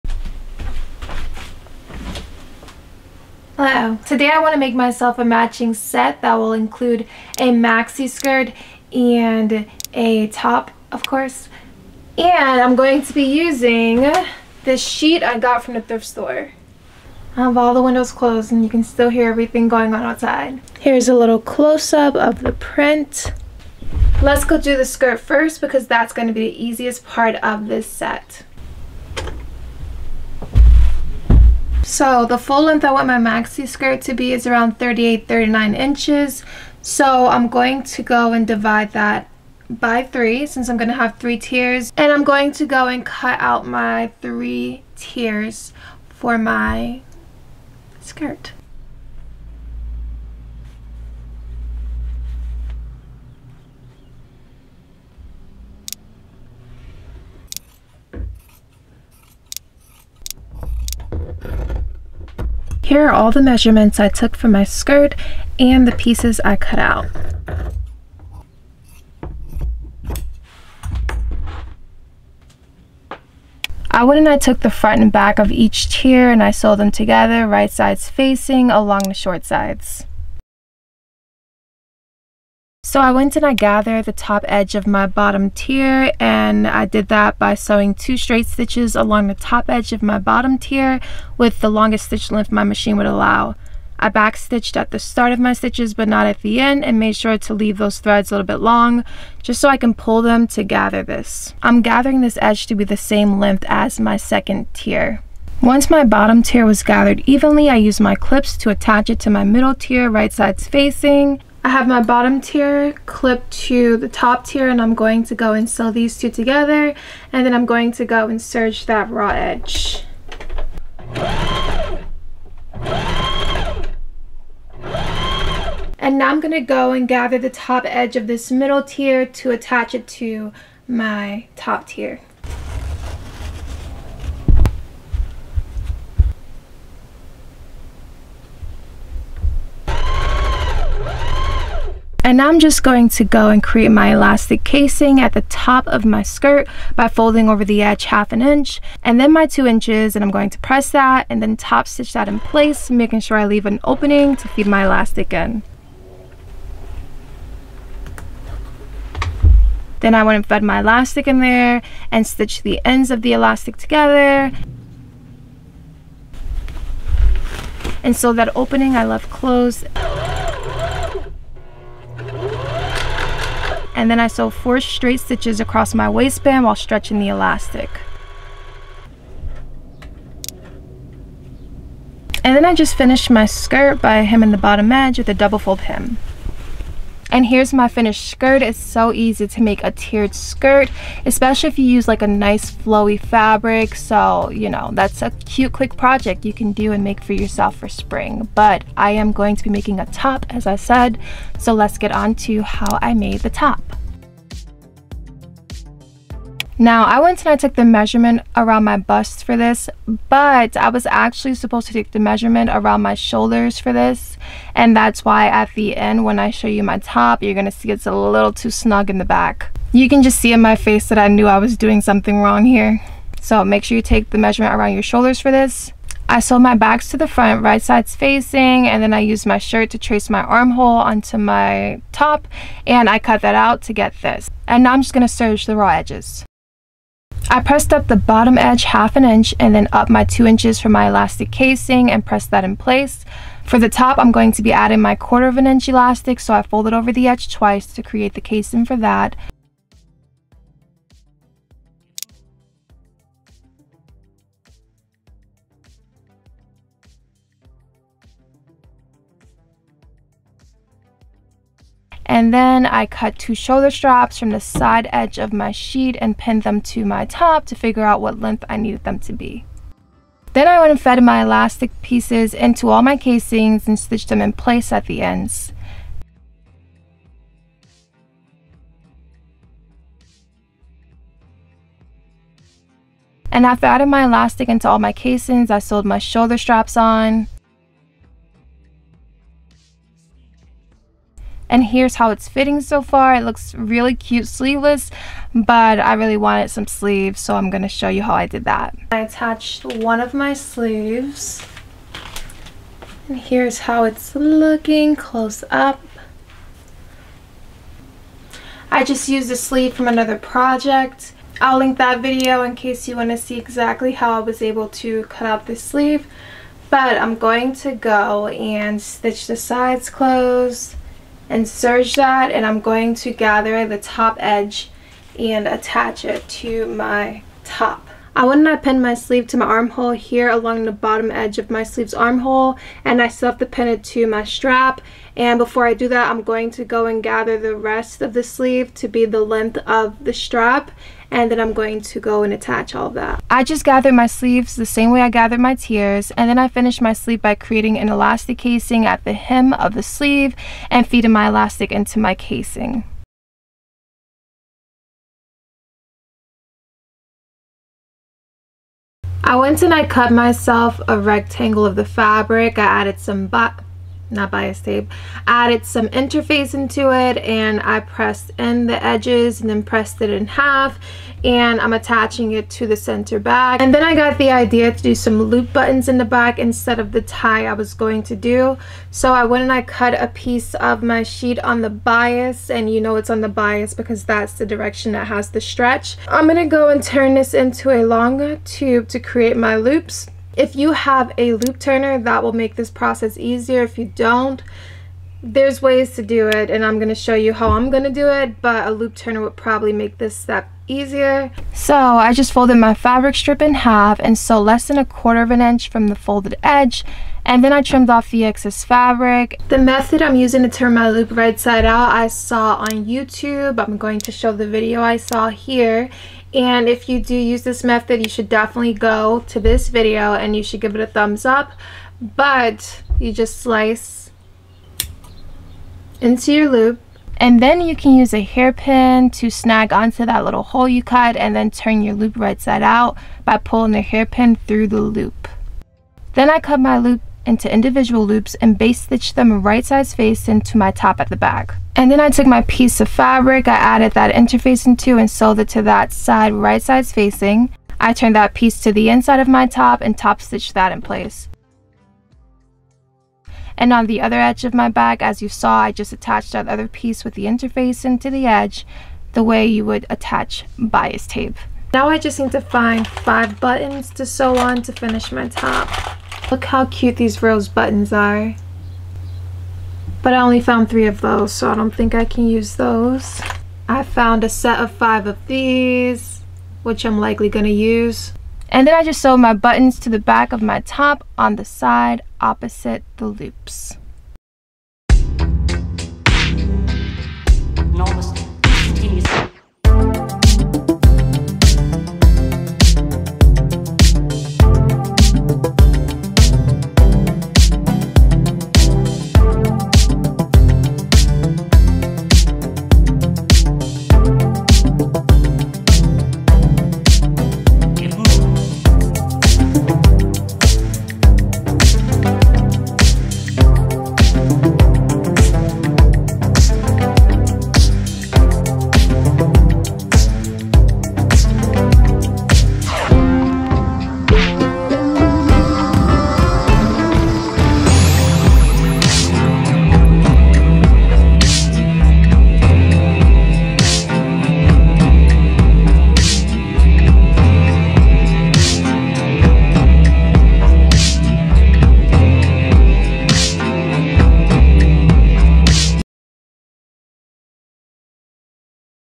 I turned a bedsheet into the CUTEST tiered skirt and top matching set! DIY tiered skirt tutorial
Hey!! In this video I show you all how I took this sheet I found at the thrift store for only $3 and turned it into this super cute DIY tiered maxi skirt and top! I love a good thrift flip and this sewing project here is a great one for spring and summer!